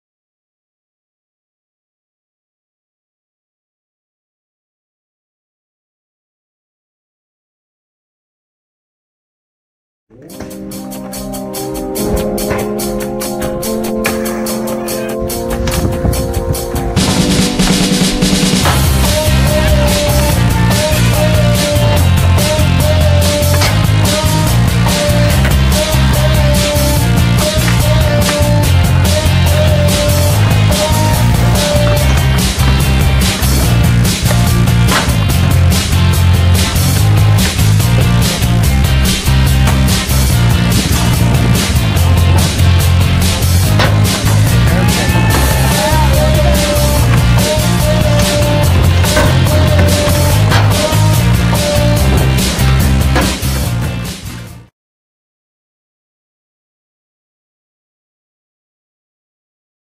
Tobyhanna Skatepark 2019 (trailer)
Just a quick preview of whats to come October 2019 .
Tobyhanna Skatepark, Pennsylvania